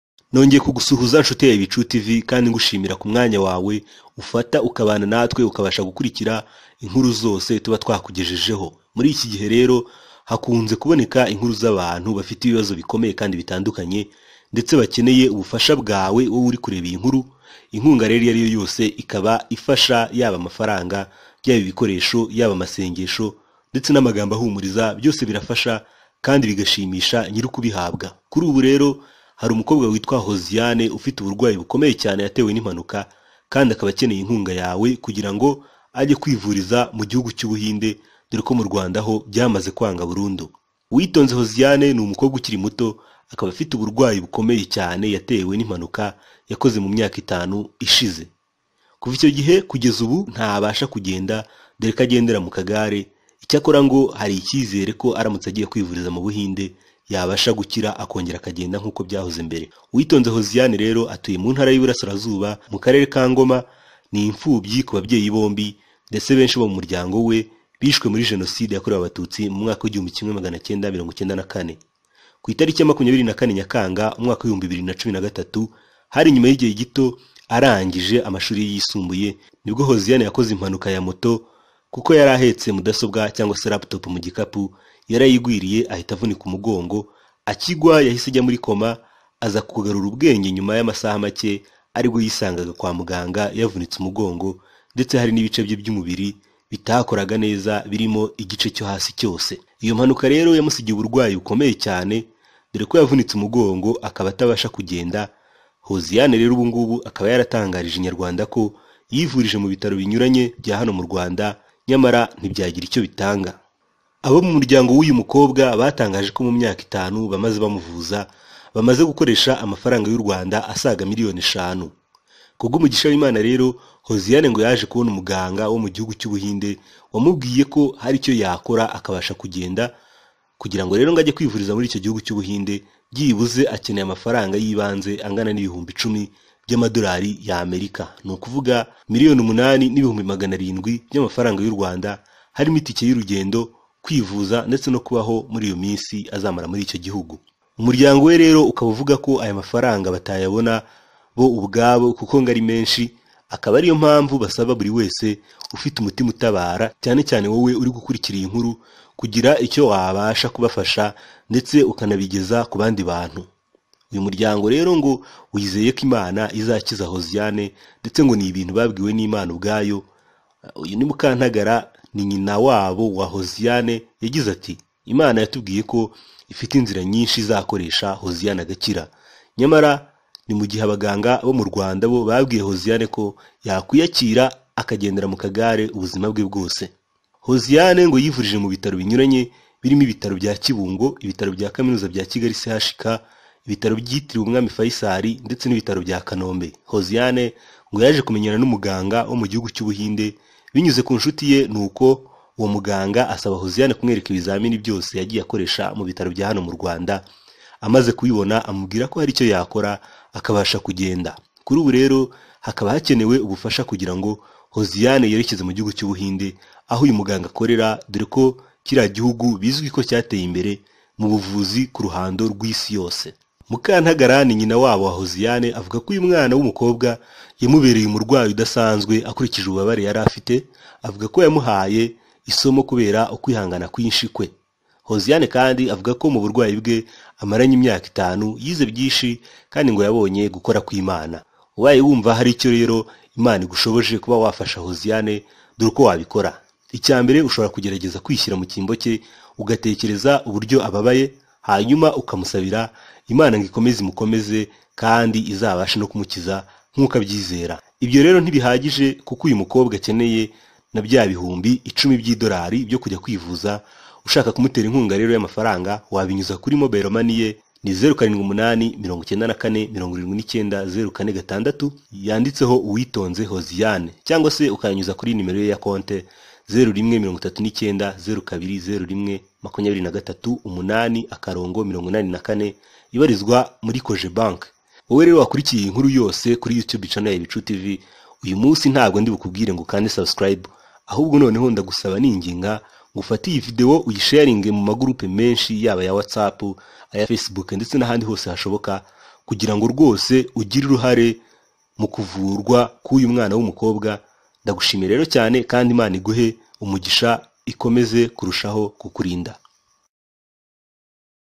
HOZIANE : AKENEYE UBUFASHA BWAWE KUGIRANGO AKIRE
Nyuma y'imyaka itanu yaratewe ubumuga n'impanuka, akeneye ubufasha bwawe ngo akire!